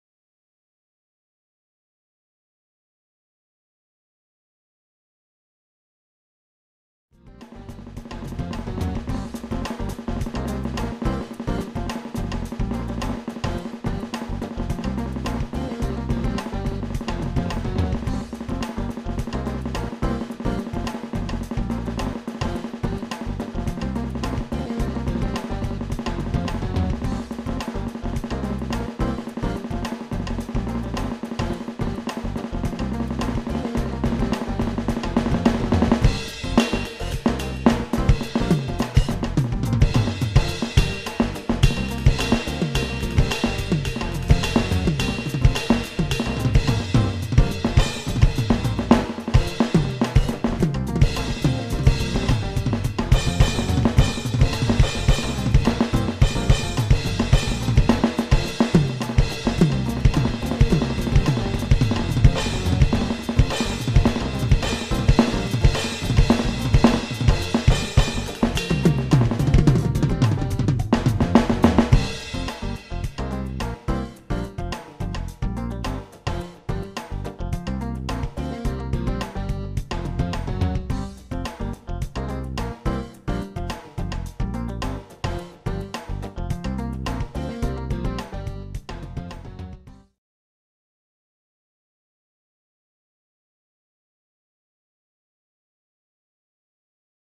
Kevin Crabb Drum Solo Triplet Singles (08/31/23)
Kevin Crabb Drum Solo Turning/Rotating The Wrist... LA Studio